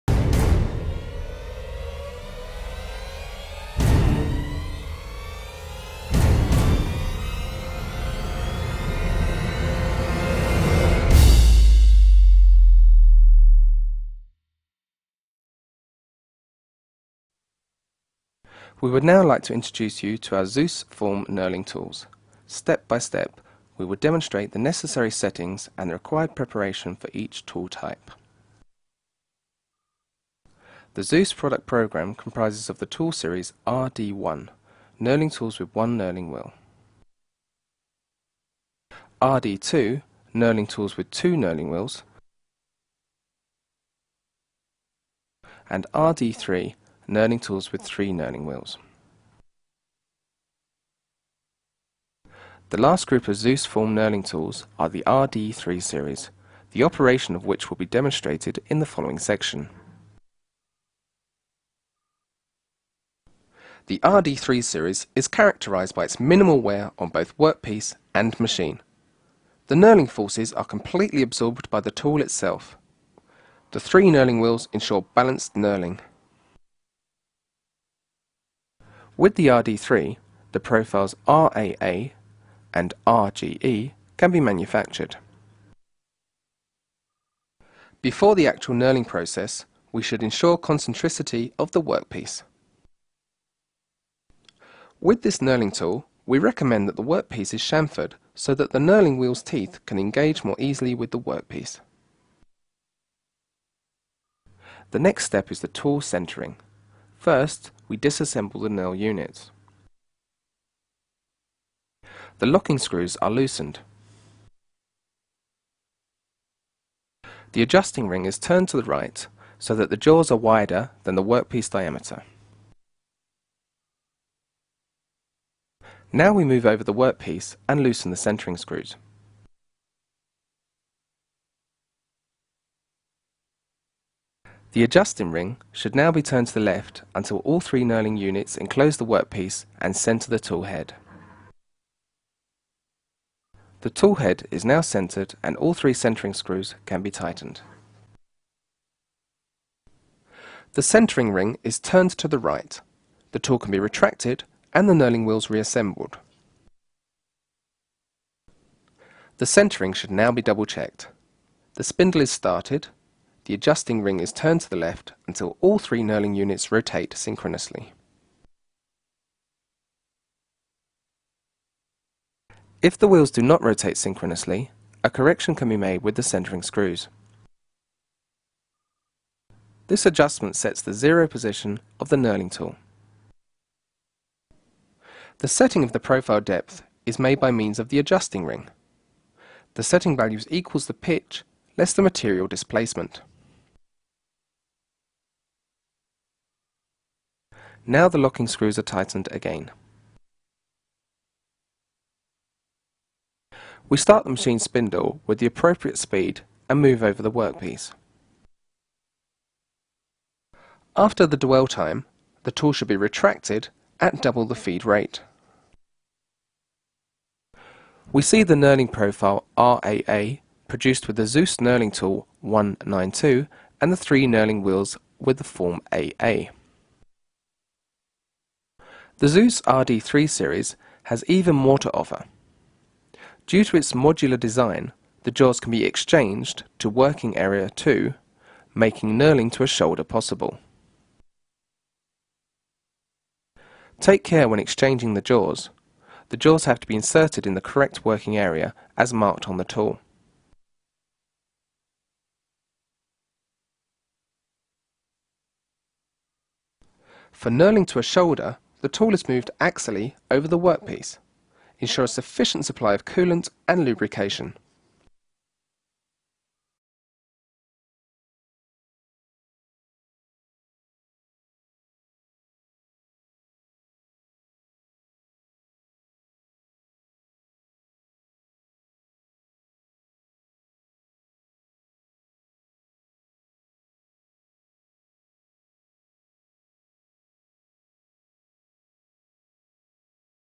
Form Knurling RD3
The RD3 Form knurling system is a safe bet on all machine types for maximum rigidity with minimal presure with a form knurling tool, has the widest range for roll diameters, RAA, RGE 30 and 45 profiles.